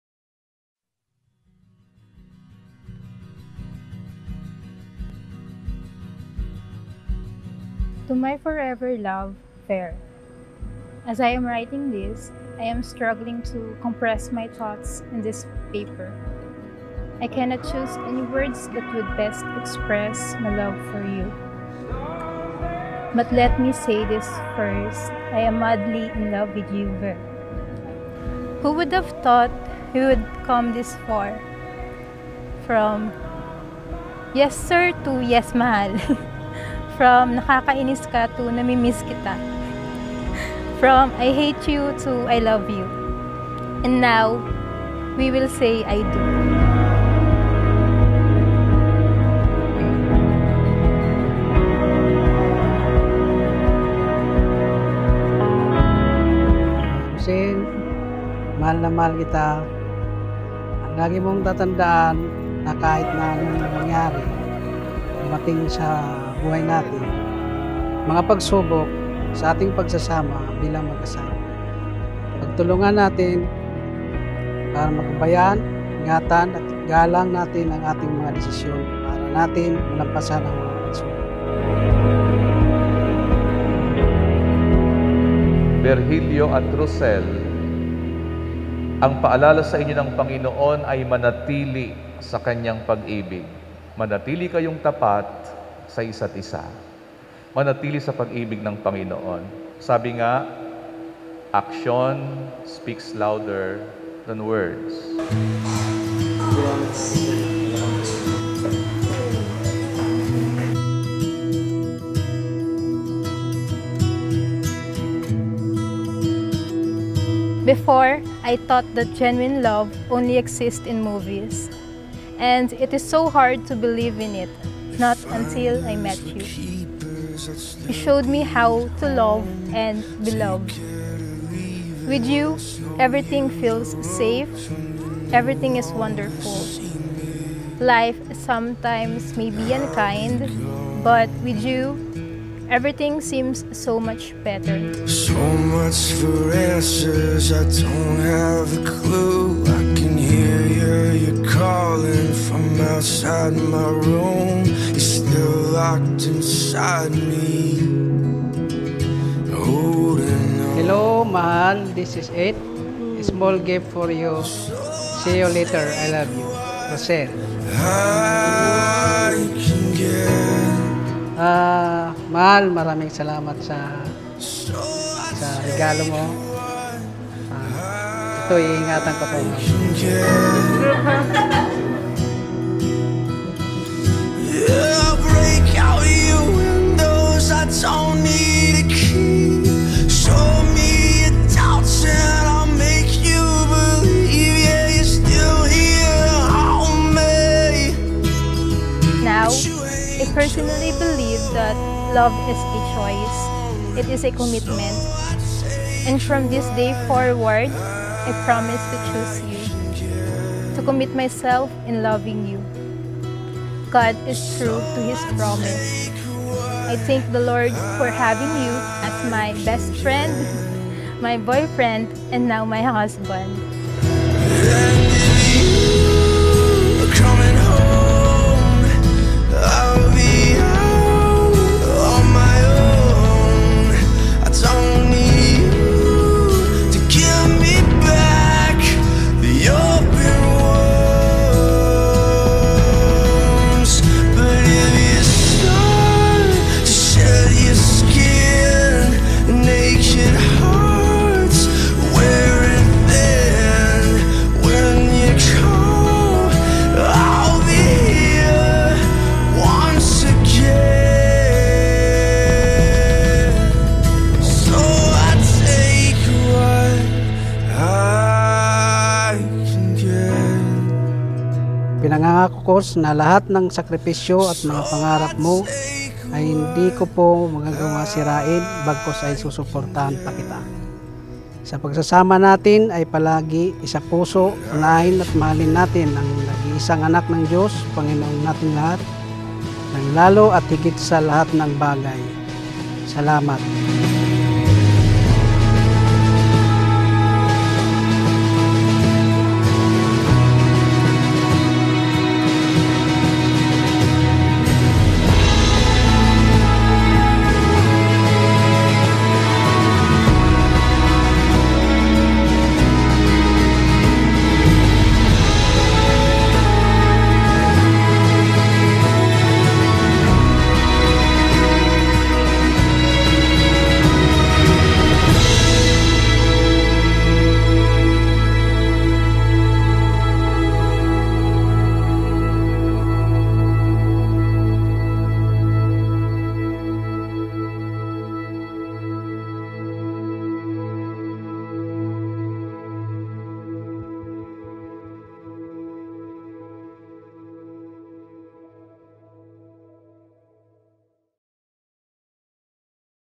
VER AND ROSEL WEDDING HIGHLIGTHS
Ver and Rosel Wedding

Thank you, Lord, for the blessings.
Thank you to everyone who has been part of our story.
Another chapter of everlasting love.

P&V : Paolo Azarcon Photography
HMUA : Hair and Makeup by: Lyndon San Antonio
Styling & Coor : La Estilo

Singer: Matthew Mayfield